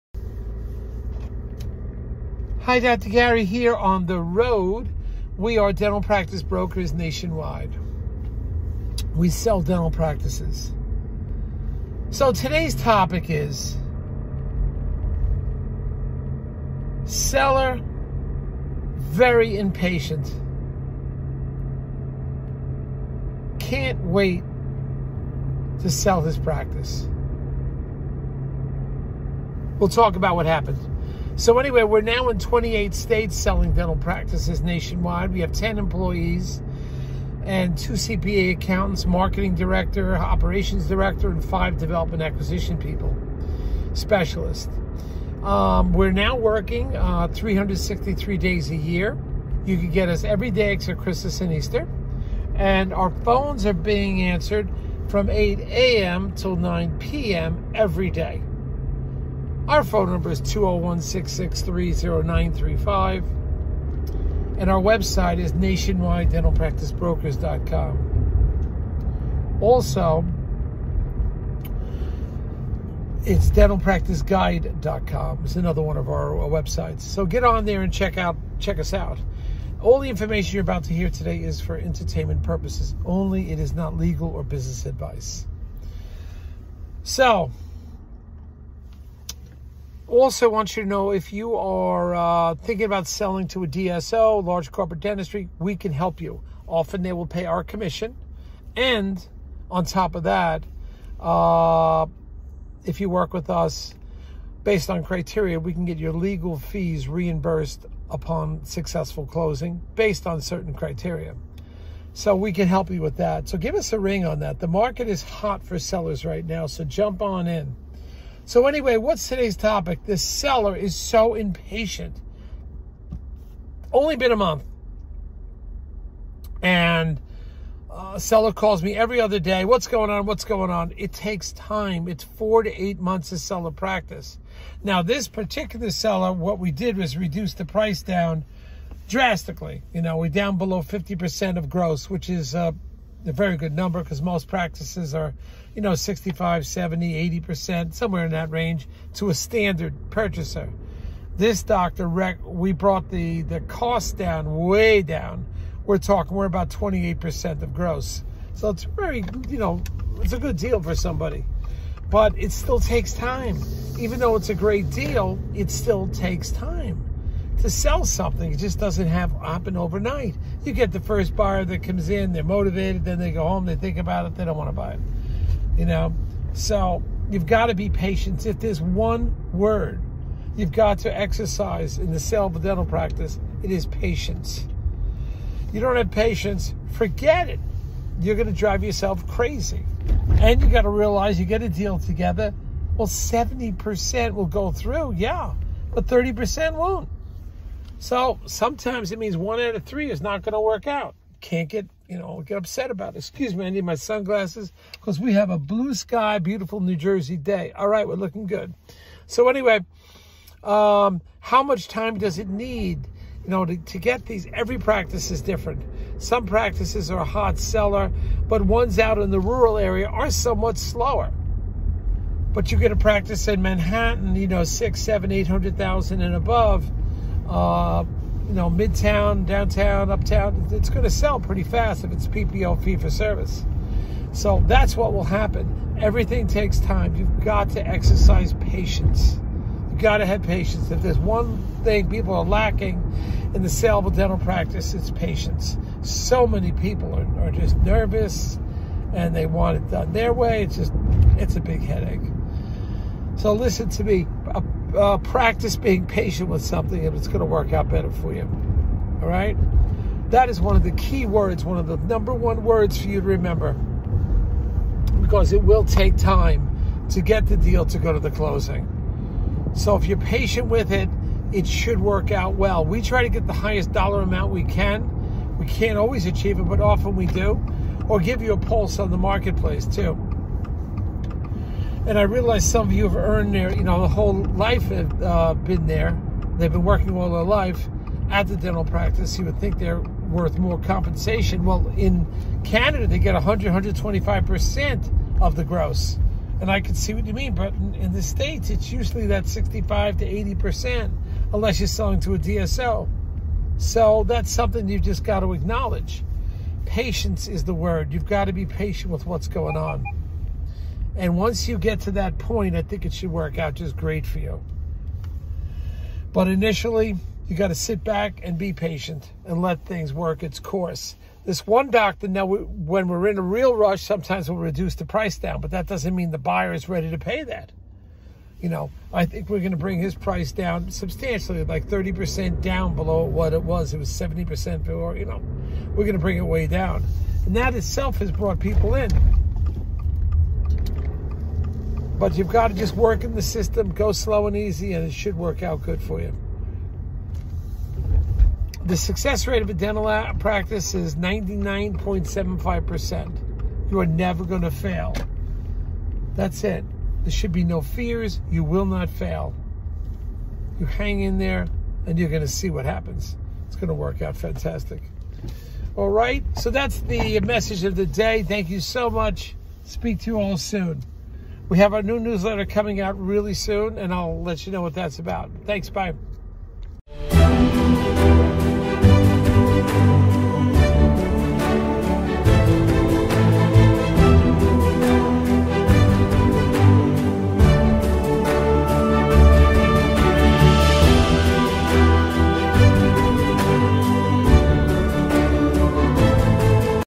When The Dental Practice Seller Is Very Impatient
In this episode, Dr. Gary will discuss When The Dental Practice Seller Is Very Impatient. To learn more about our company, please visit: dentalpracticeguide.com